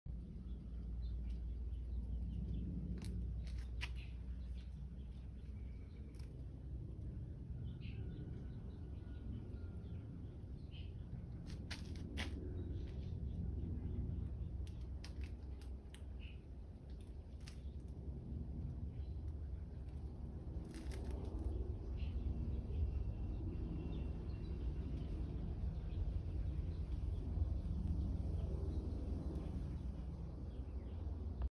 butterly&zinnia
A beautiful swallowtail butterfly checking out my zinnias